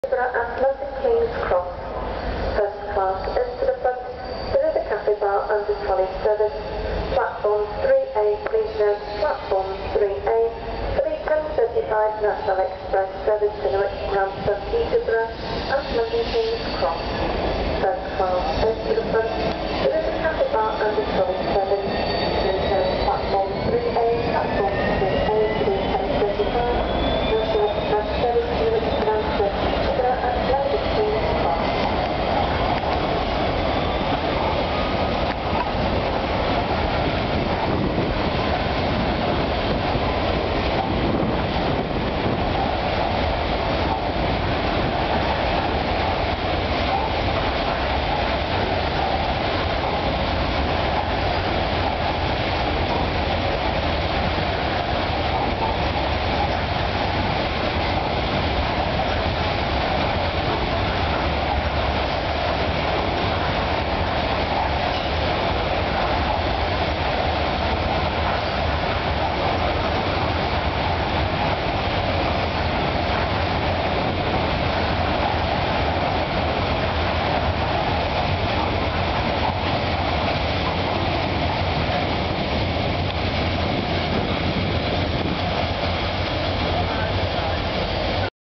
DB Schenker 66171 passing Doncaster heading north on a coal train 26th June 2009
DB Schenker heading north passing Doncaster on a coal train